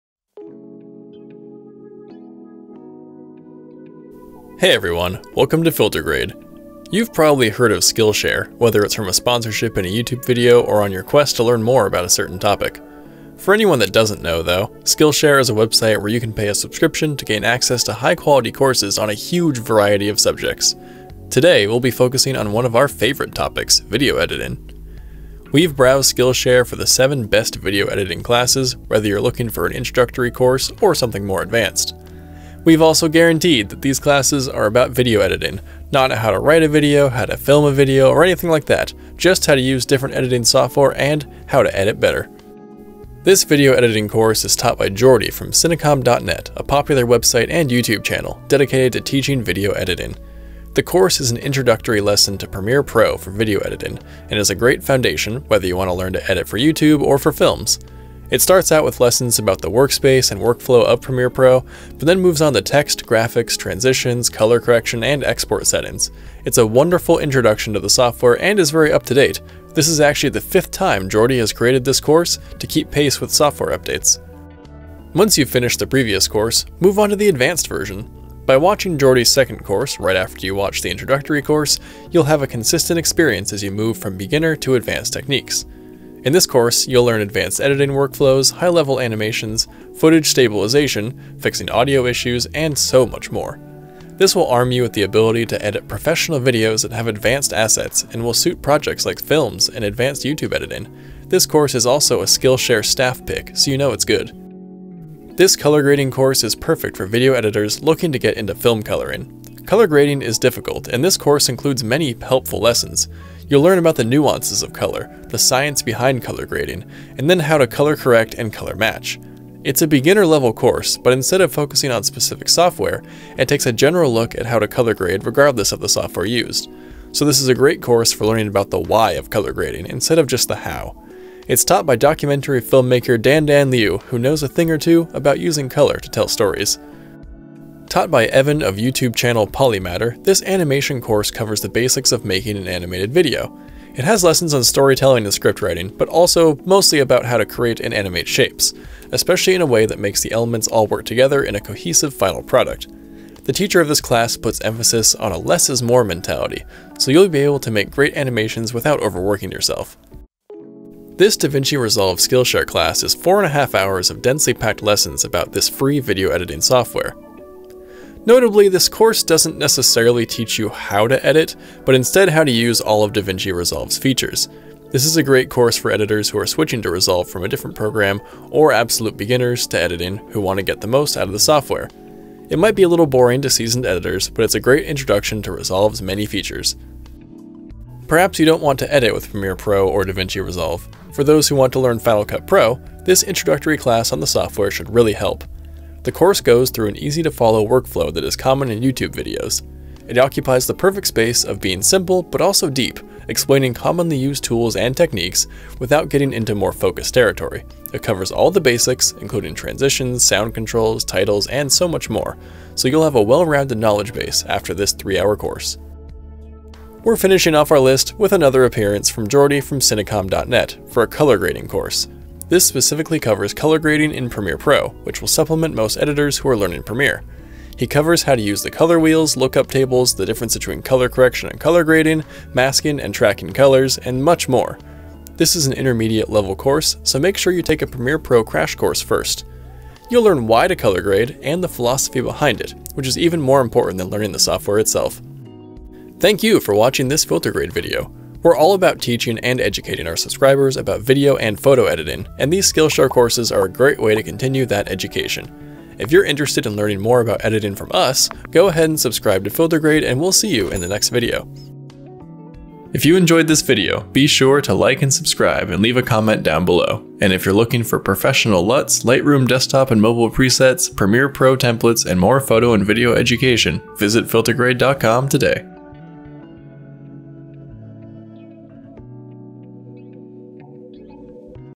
7 of the Best Video Editing Classes on Skillshare
You’ve probably heard of Skillshare, whether it’s from a sponsorship in a YouTube video or on your quest to learn more about a topic. For anyone that doesn’t know, Skillshare is a website where you can pay a subscription to gain access to high-quality courses on a huge variety of topics. Today we’ll be focusing on one of our favorite topics, video editing. We’ve browsed Skillshare for the 7 best video editing classes, whether you’re looking for an introductory course or something more advanced. We’ve also guaranteed that these classes are about video editing - not how to write a video, how to film a video, or anything like that. Just how to use different editing software and how to edit better.

Video Editing with Adobe Premiere Pro for Beginners by Jordy Vandeput

Advanced Video Editing with Adobe Premiere Pro 2020 by Jordy Vandeput

Color Grading for Filmmaking: The Vision, Art, and Science by Dandan Liu

DaVinci Resolve 16 - The Complete Video Editing Course by Patrick Inhofer

Video Editing with Final Cut Pro X - From Beginner to YouTuber by Ali Abdaal

Premiere Pro Lumetri 2020: Color Correct & Color Grade like a Pro by Jordy Vandeput

Follow FilterGrade!